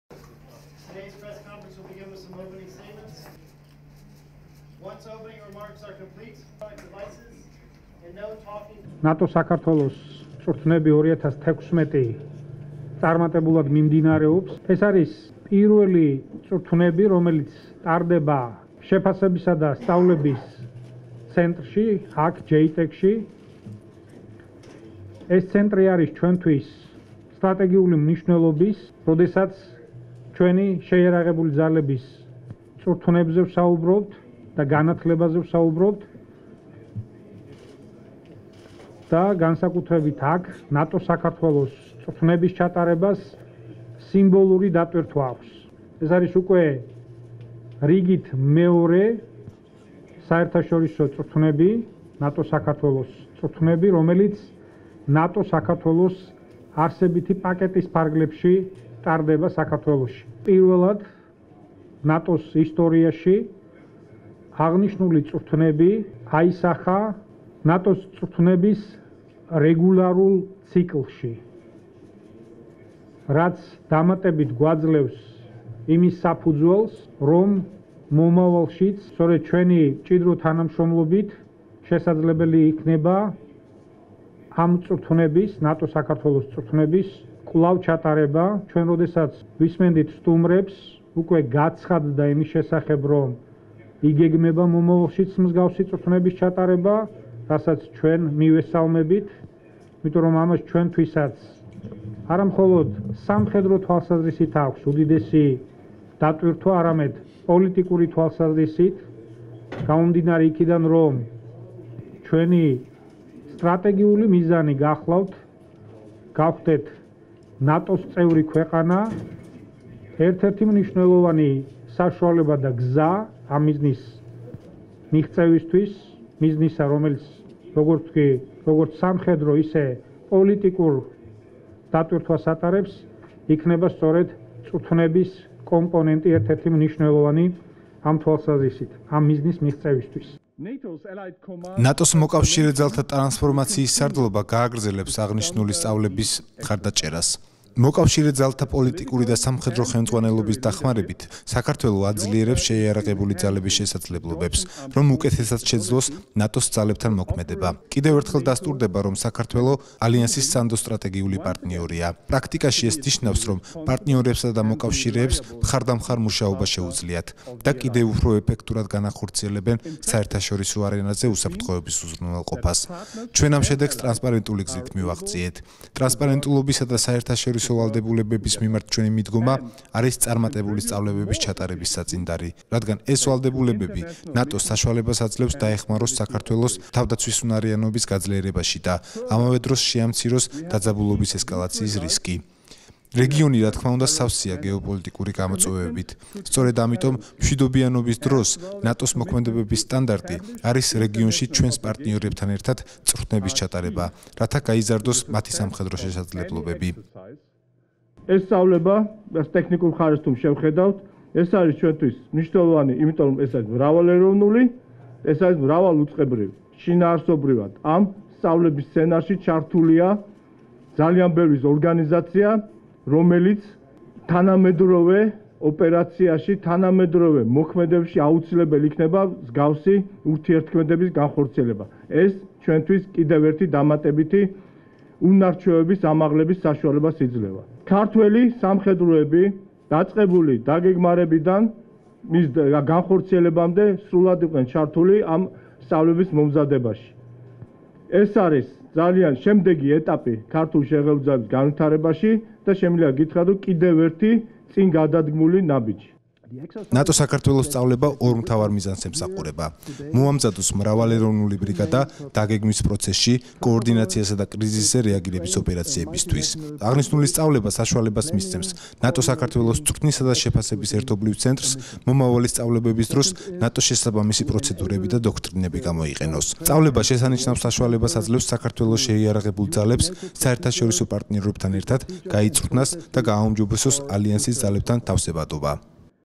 ადმირალი ნილსენი: "საქართველო ნატოს სანდრო სტრატეგიული პარტნიორია"
დღეს "ნატო-საქართველოს სწავლების 2016" მიმდინარეობის გასაცნობად ნატო-საქართველოს წვრთნებისა და შეფასების ერთობლივ ცენტრს (JTEC) 130-ზე მეტი მაღალი დონის წარმომადგენლისგან შემდგარი დელეგაცია ეწვია.